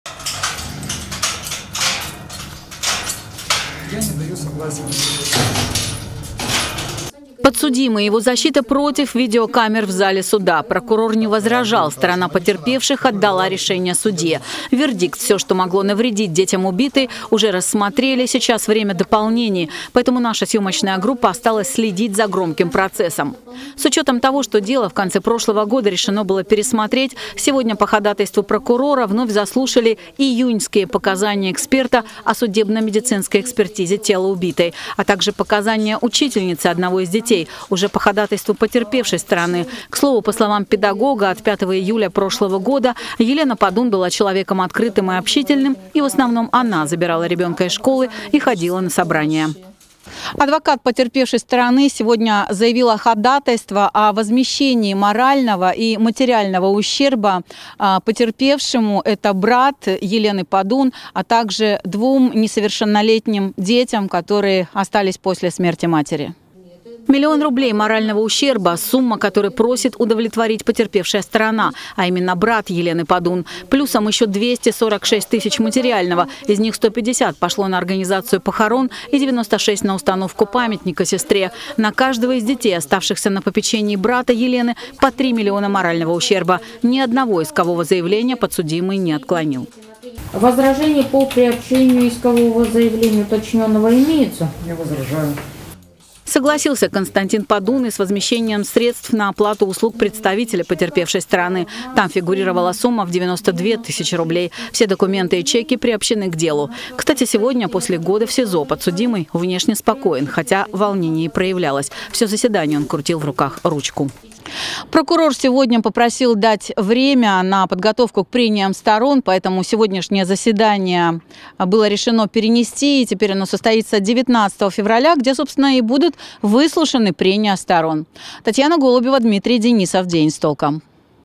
Родственники погибшей Елены Падун запросили миллион рублей компенсации
В открытом режиме. Впервые после долгого перерыва на очередном заседании по делу Константина Падуна,  которого обвиняют в убийстве бывшей супруги в январе прошлого года, разрешили присутствовать журналистам, а именно съемочной группе нашего телеканала. Правда, стороны процесса придерживались разного мнения по поводу присутствия прессы в зале суда.  Подробности заседания в нашем сюжете.